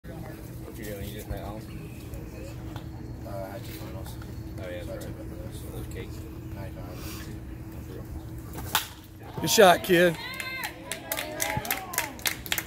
Brandon Mercer 15u , usa scout national. 06-2023
15u travel ball. usa scout national. East cobb. rip up the middle